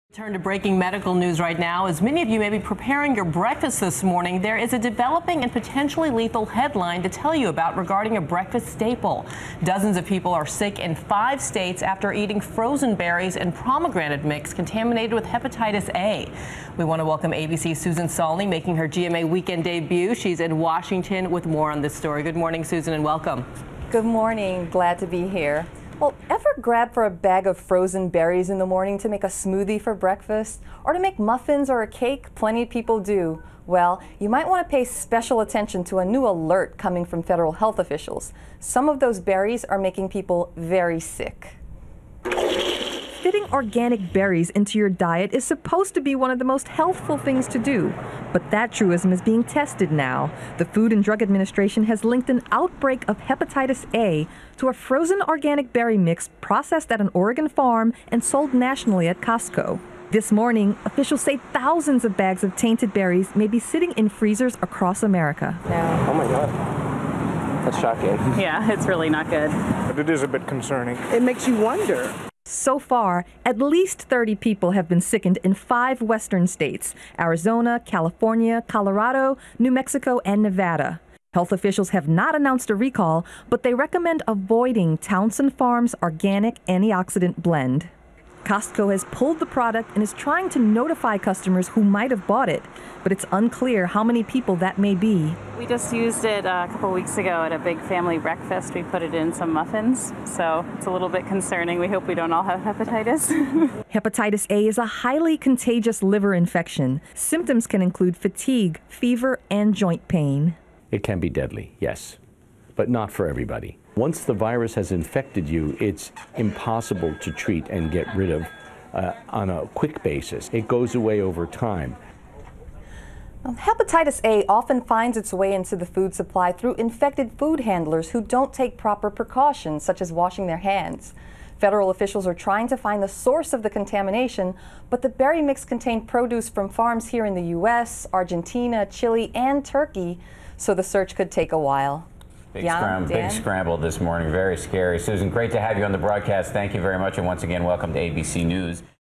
Frozen Berries Contaminated With Hepatitis A
Dozens of people are sick across five states after consuming the frozen fruit.